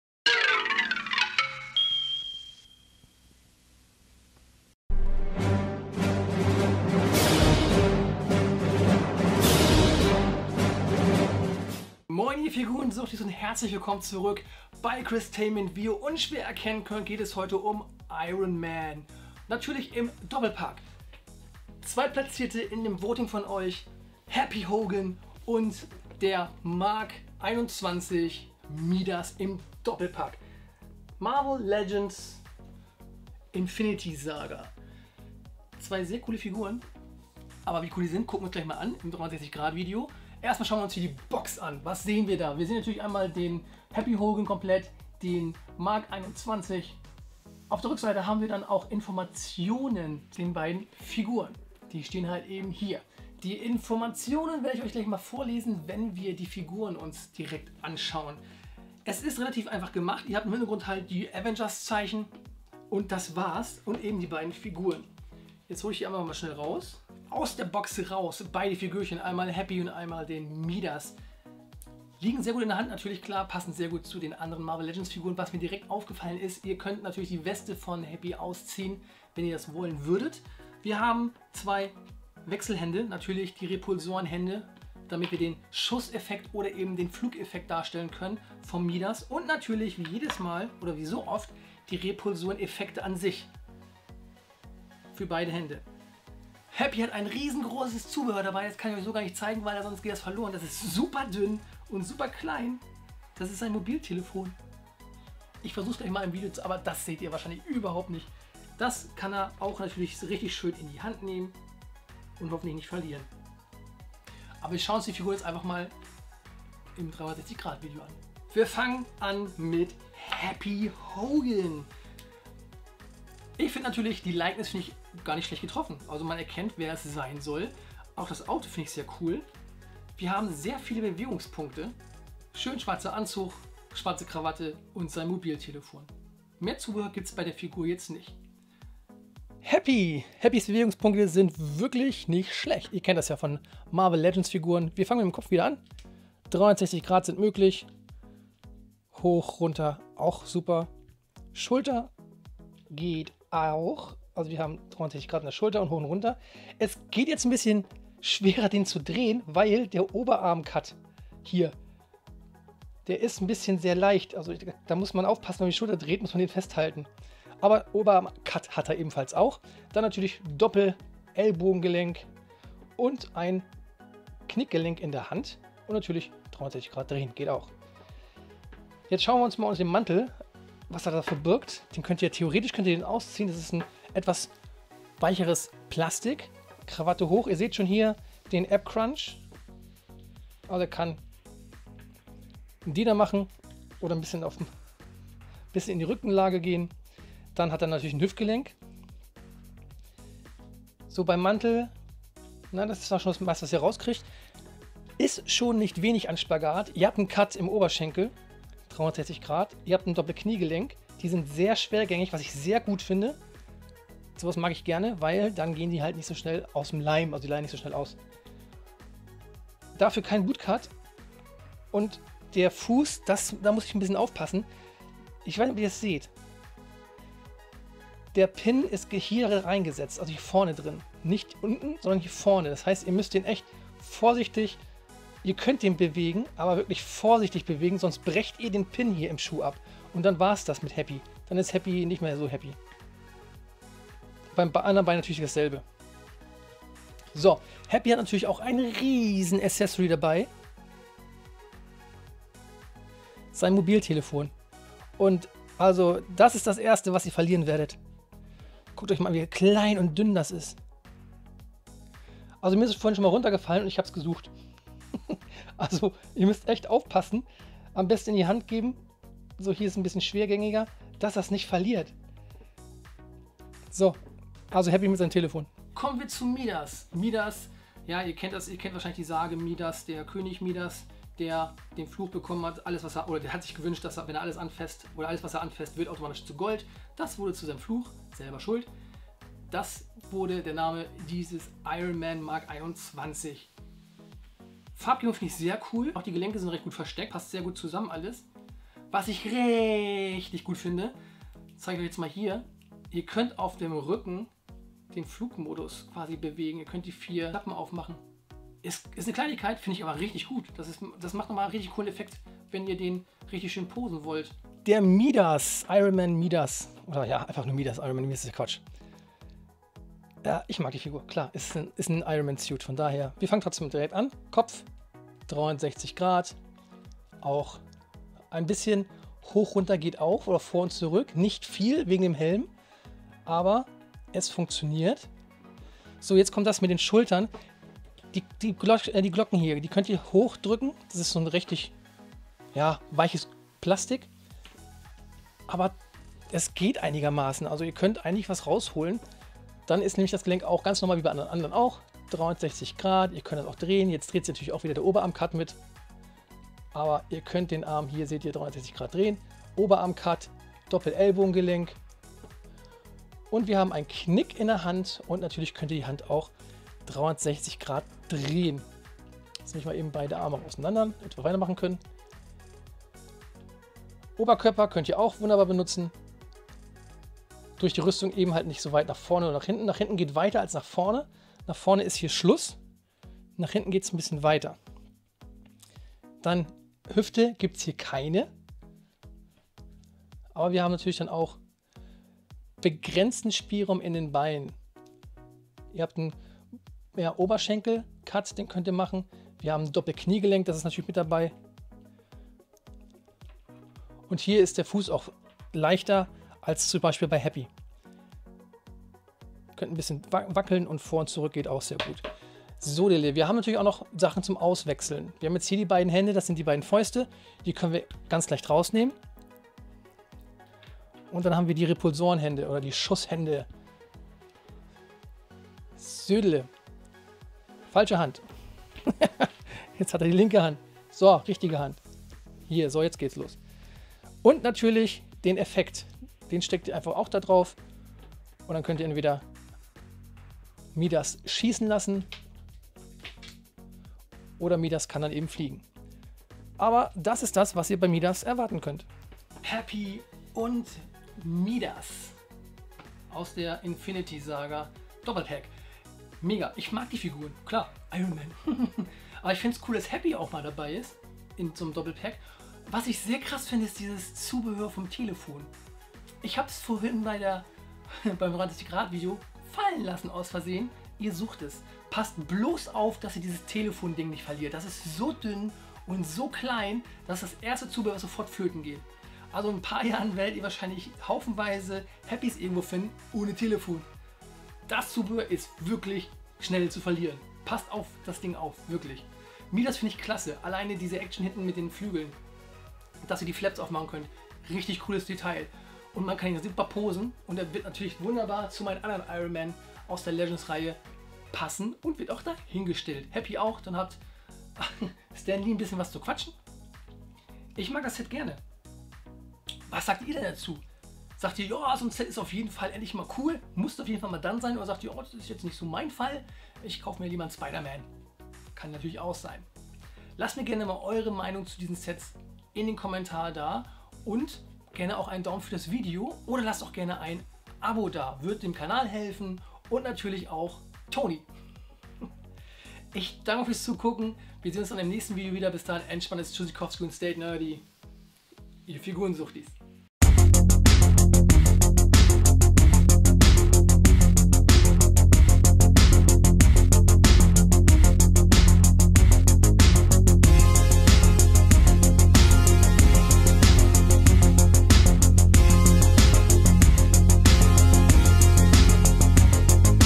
IRON MAN 3 The Infinity Saga 2 Pack MKXXI & HAPPY HOGAN
MARVEL LEGENDS "THE INFINITY SAGA"  2 PACK

Happy Hogan + Midas MKXXI  (21)

Der neue Sicherheitschef von Stark Industries wird in der Mitte des Gefechts überrascht, als Iron Man einer neuen Bedrohung gegenübersteht.
Mark 21, Codename "Midas", ist ein von Stark entworfener, voll ausgestatteter Anzug für große Höhen mit einer angereicherten, goldenen Titanlegierung.

Danke an Hasbro und Alex für dieses tolle Pack !

Logo Artwork & Intro by Chriss Tainment